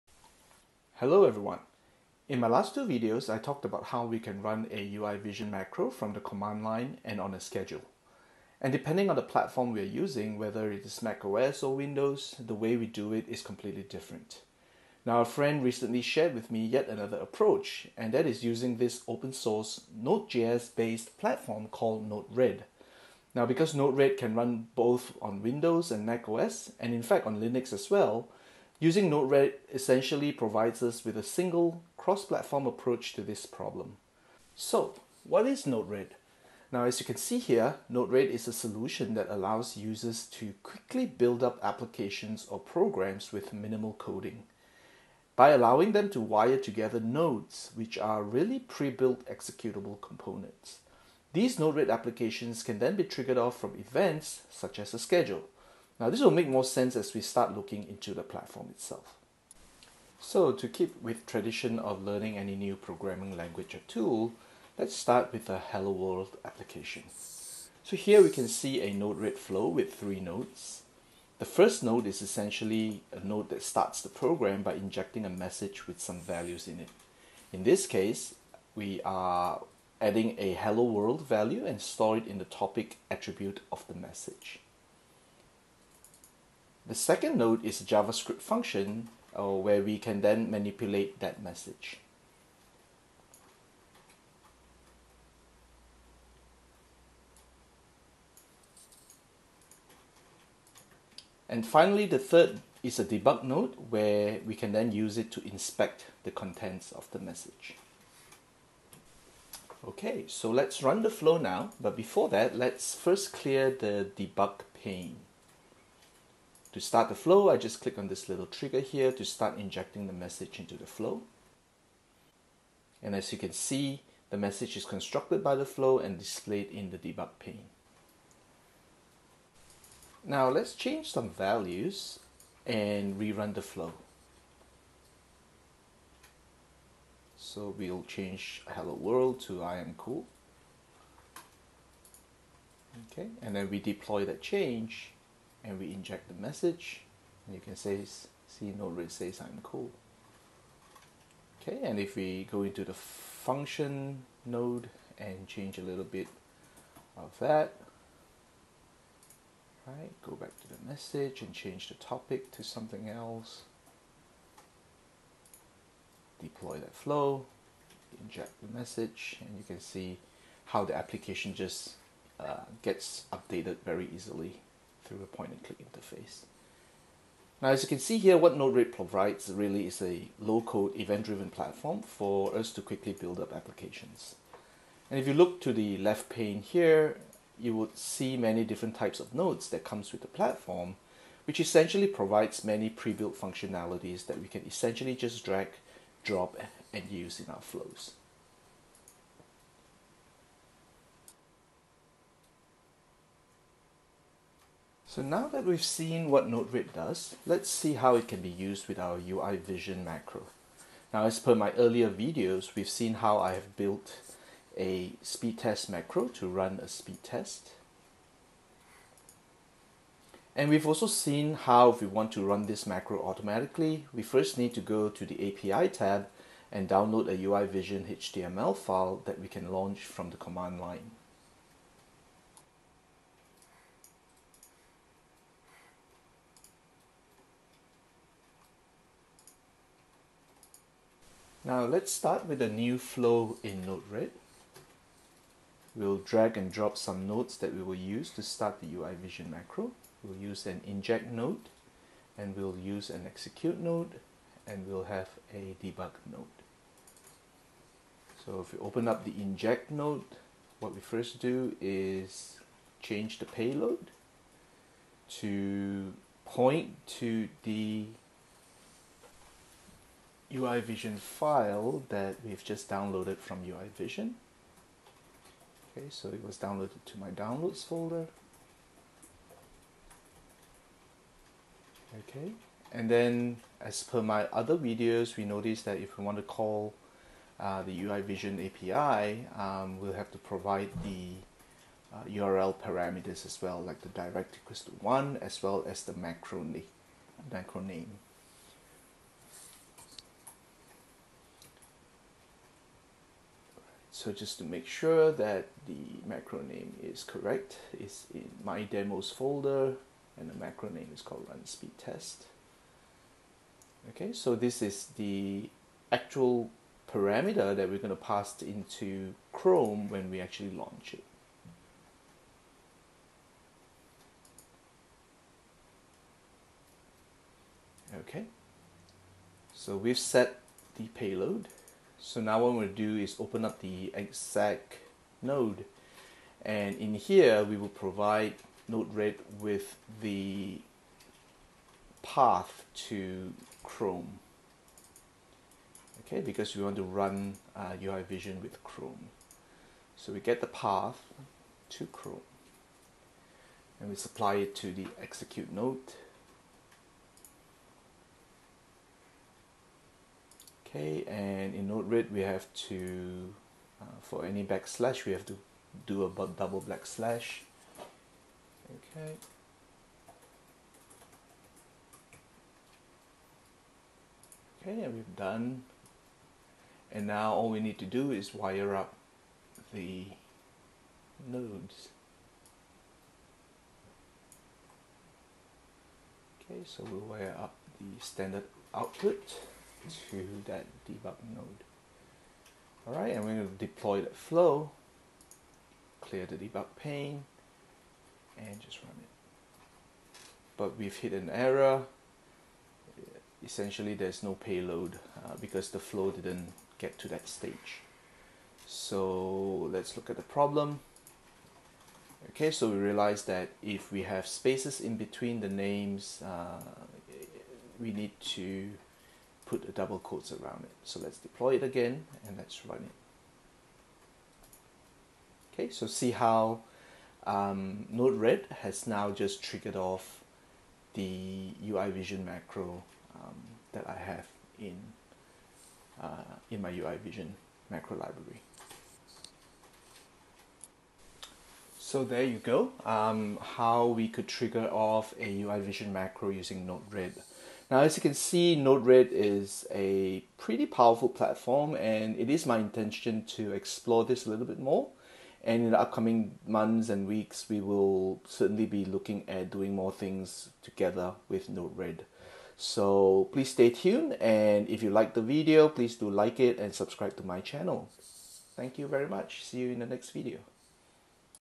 Running UI.Vision Macros with Node-Red
Note: The first 3:25 mins of the video is a high level introduction of Node-Red. If you want to just see how to use Node-Red with UI.Vision, just go straight to the 3min 25second mark.

This video shows how we can use a low-code, event-driven platform called Node-Red to trigger off UI.Vision macros. Because Node-Red is cross platform, that means it presents a single approach to running UI.Vision macros no matter you are using Windows, macOS or Linux. Node-Red is also a powerful platform that can do a lot more than just triggering off the macros. This is the first of a series of videos where I will be exploring using both these platforms to do geeky automation stuff. Enjoy!